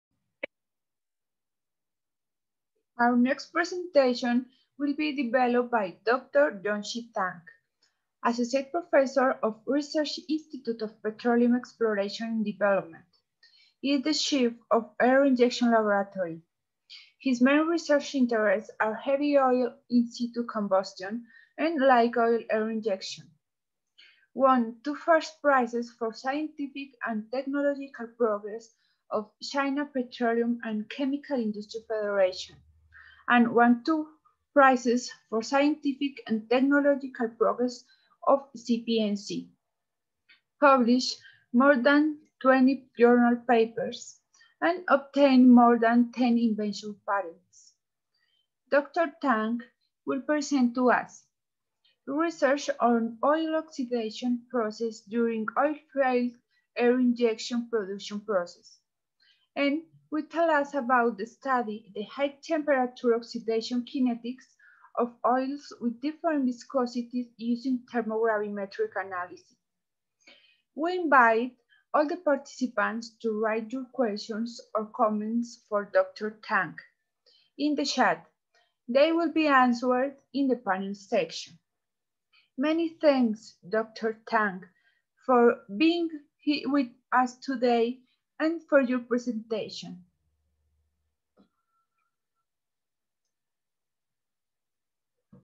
Dr. Junshi Tang - Research on oil oxidation process during oilfield air injection production process
High temperature oxidation (HTO) kinetics is very important for the propagation stability and combustion front temperature during in-situ combustion. This paper studied the HTO kinetics of different oils.  Thermogravimetric analysis was used to study the HTO kinetics of oils with different viscosities. DSC, TGA and ARC were used in this paper. Oil samples with different viscosities were taken from air injection pilots of CNPC. Oxidation heat measurements of different samples and SARA fractions were carried out. Influence of pressure and oxygen fraction on HTO and LTO reaction heat was studied.
Oxidation test showed that oil samples with different viscosities have similar LTO reaction temperature range and HTO reaction temperature range. Heavy oil samples also have high LTO heat effect. The measured reaction heat of HTO by unit mass of coke is large than that of LTO by unit mass of oil. The fuel for HTO of light oil was much smaller compared with heavy oils, which made the peak reaction rate smaller than heavy oils. Kinetics parameters determined by single test curve method was not accurate due to the influence of determination process of reaction order. Iso-conversional methods can get the intrinsic kinetic parameters. Although the viscosities and compositions of oils were different, the HTO activation energies were more or less the same. The activation energy range for three oils was 130-154 kJ/mol. Kinetic cell and effluent gas analysis technique were used in this study. Fuel amount and fuel composition were calculated by the effluent CO2, CO and O2 concentrations. The results showed that fuels of HTO reactions were coke with very low hydrogen content. Heavy oil produced more coke than light oil. Although the viscosities and compositions of oils were different, the fuels for HTO reaction were similar. So, TGA analysis showed similar HTO kinetics for different oils.

Dr. Junshi TANG, associate professor of Research Institute of Petroleum Exploration and Development(RIPED), is the chief of air injection laboratory. His main research interests are heavy oil in-situ combustion and light oil air injection. Won two first prizes for scientific and technological progress of China Petroleum and Chemical Industry Federation, and one two prizes for scientific and technological progress of CPNC. Published more than 20 journal papers, obtained more than 10 invention patents. Dr. Tang will present to us "Research on oil oxidaiton process during oiflied air injection production process" and will tell us about the study the High temperature oxidation kinetics of oils with different viscosities using thermogravimetric analysis